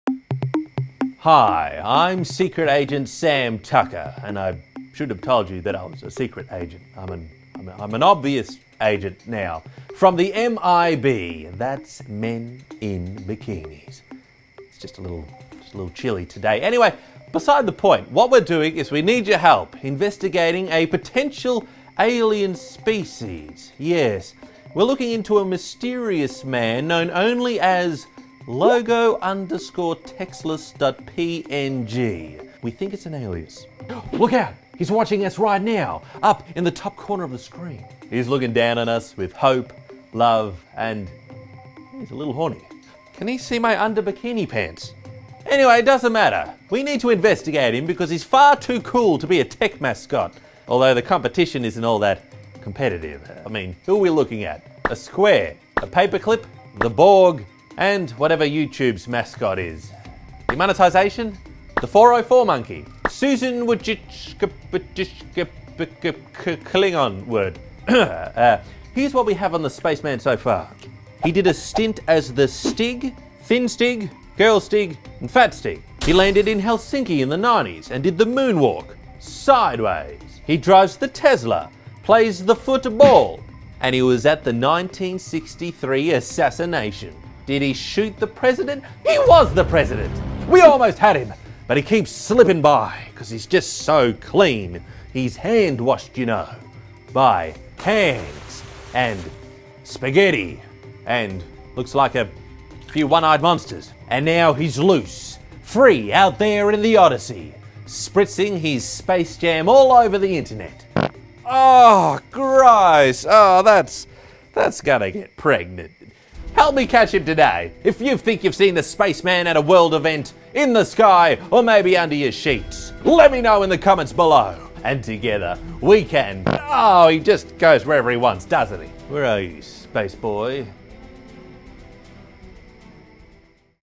Sam's Odysee: Who Is The Spaceman? (Episode 2)
In this episode, Sam explores one of Odysee's biggest mysteries:

Who is the Spaceman?

Share any theories you have in the comments because we really don't know who he is either